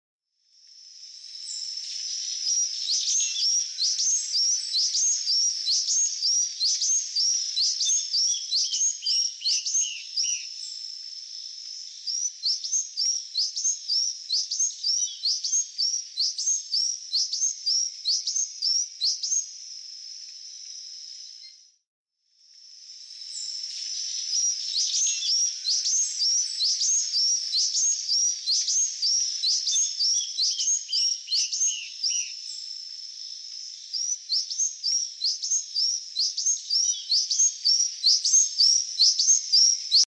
Buff tailed sicklebill hummingbird (Eutoxeres condamini) sound - Call and song
Buff tailed sicklebill hummingbird (Eutoxeres condamini) sound - Calling and singing. The buff-tailed sicklebill (Eutoxeres condamini) is a species of hermit hummingbird from the lower Andes and adjacent west Amazonian lowlands from southern Colombia and northern Ecuador to Peru and Bolivia.

Scientific classification:
Kingdom:  Animalia
Phylum:     Chordata
Class:        Aves
Order:        Apodiformes
Family:      Trochilidae
Genus:      Eutoxeres
Species:   E. condamini